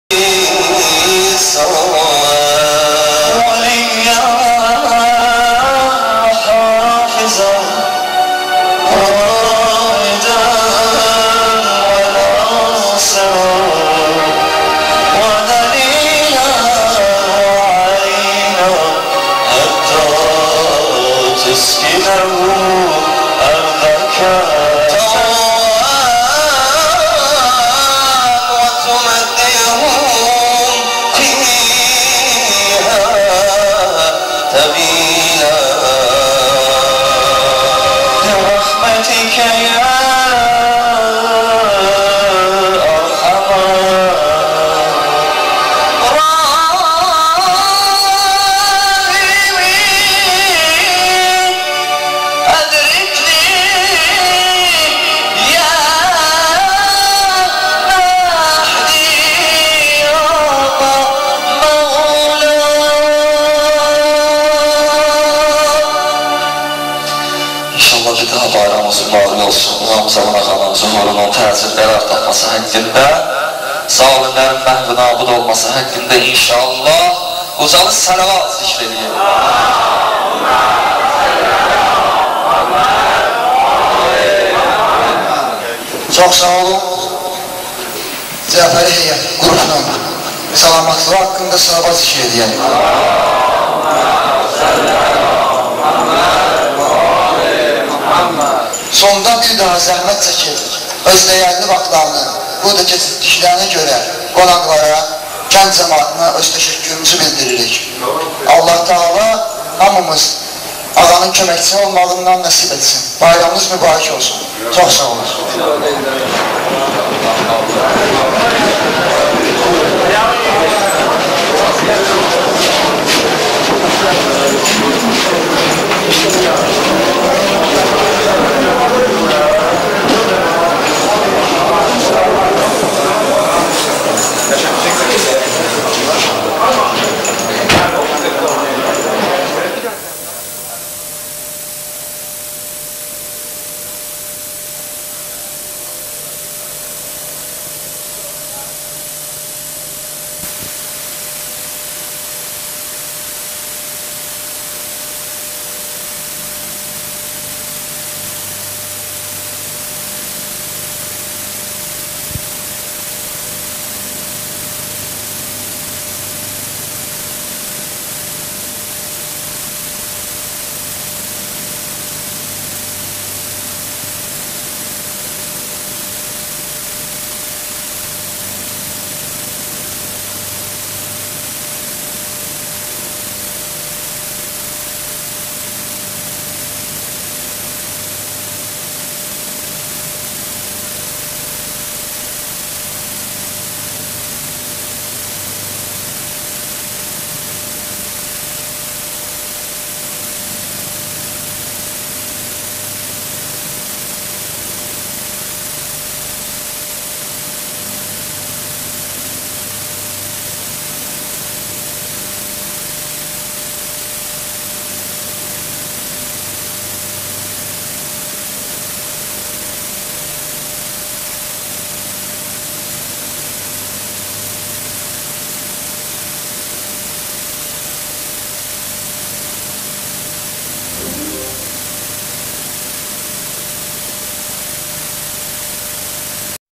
Nardaran_mədəniyyət evi_2004-cü il_Həzrət Məhdinin (ə.f) mövludu_4-cü hissə
Nardaran gənclərinin təşkilatçılığı ilə keçiriliən Həzrət Məhdinin (ə.f) mövlud mərasimi. 30 sentyabr_2004-cü il